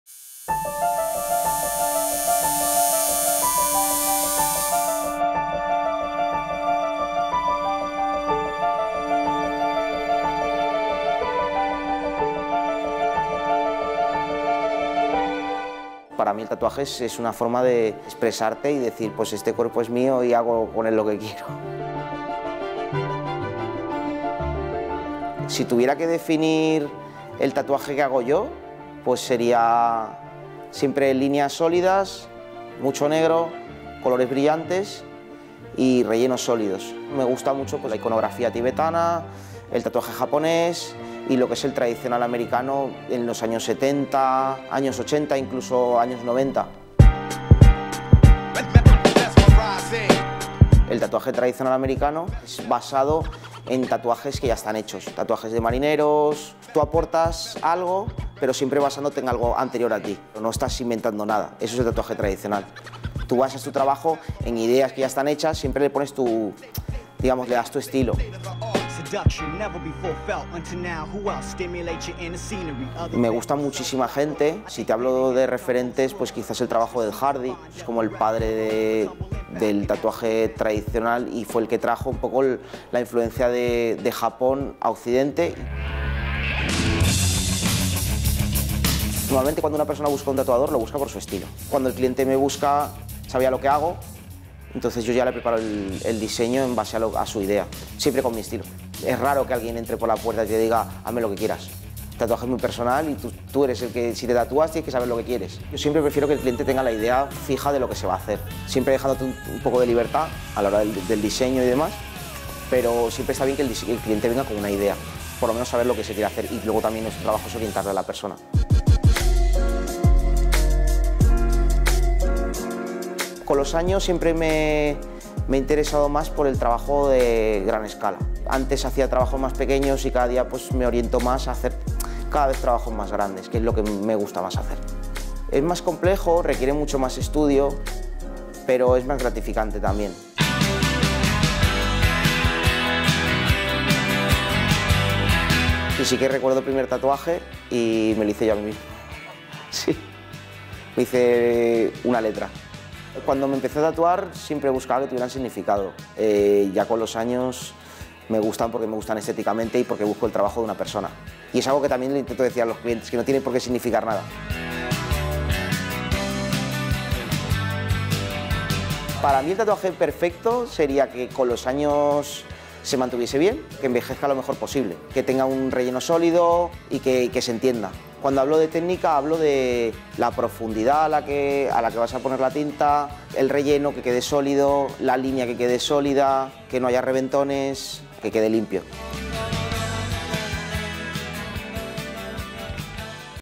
Mauricio Noguera, un estilista del tatuaje
Mauricio Noguera es un artista de la piel y la tinta. Sus creaciones buscan siempre la perfección estética, pero también la expresión artística e impregnar la profundidad de la historia del tatuaje.